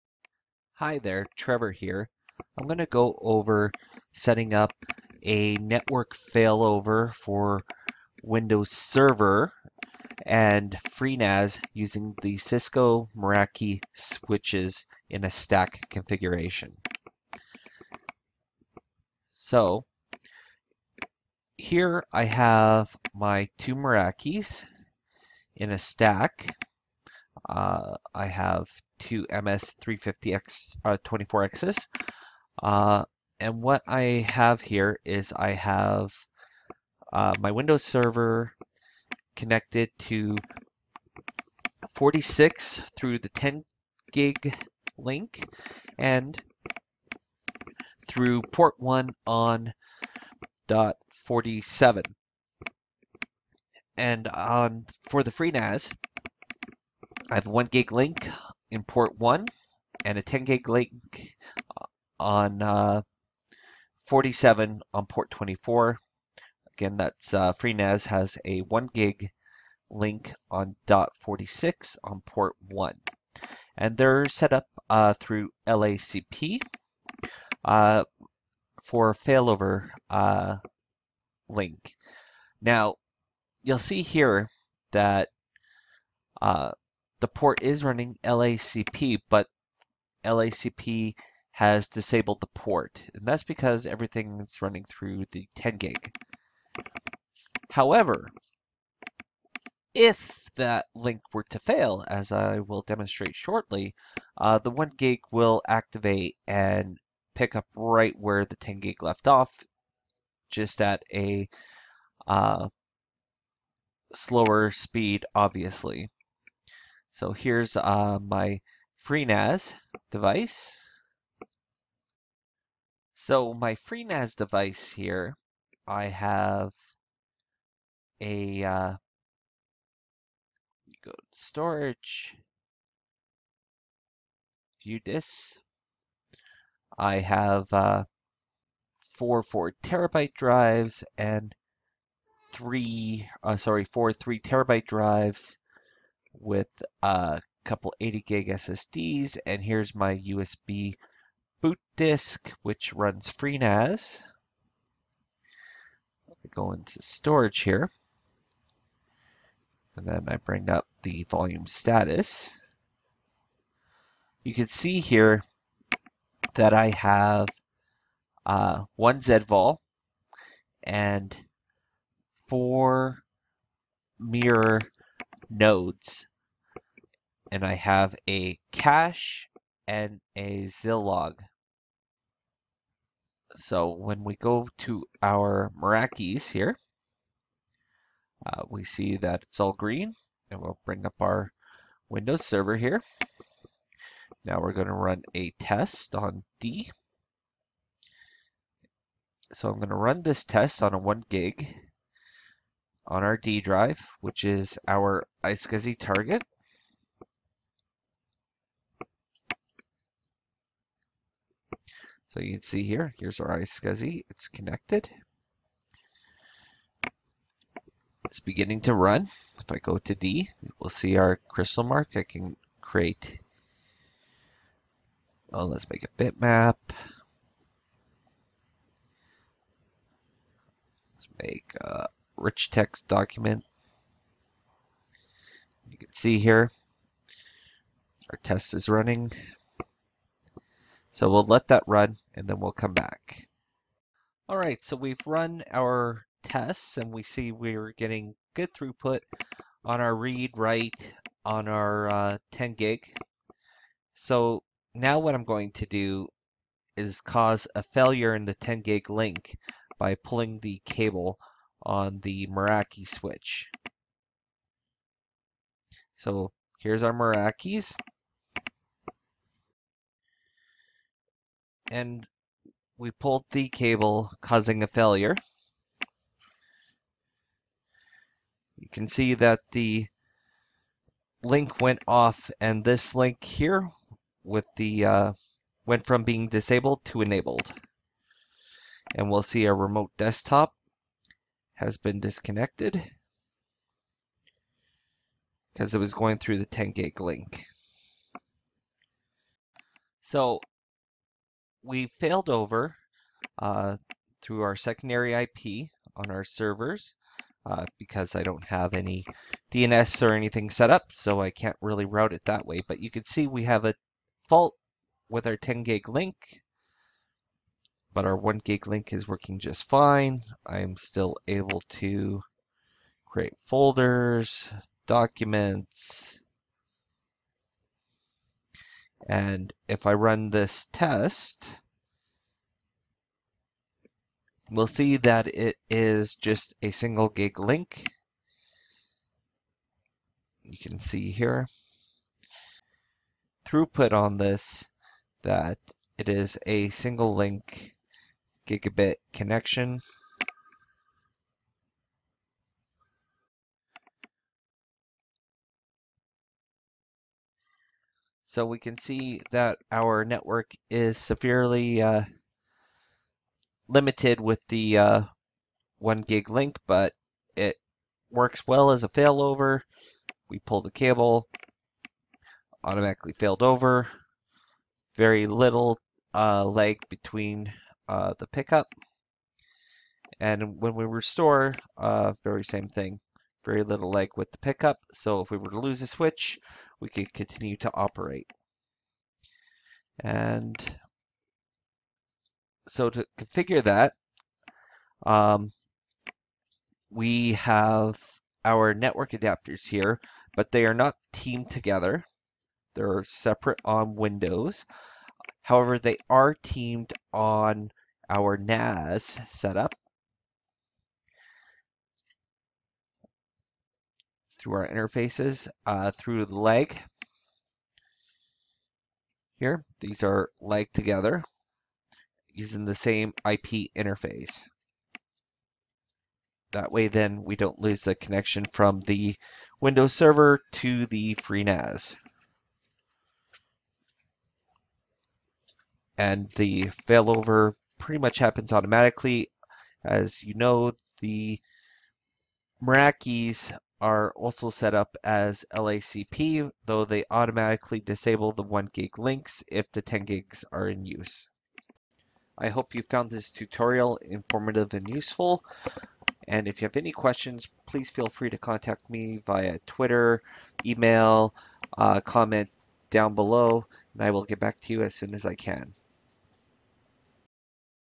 Configuring HA for Windows Server and a FreeNAS iSCSI target using Cisco Meraki Switches
Configuring High Availability for Windows Server and a FreeNAS iSCSI target using Cisco Meraki Switches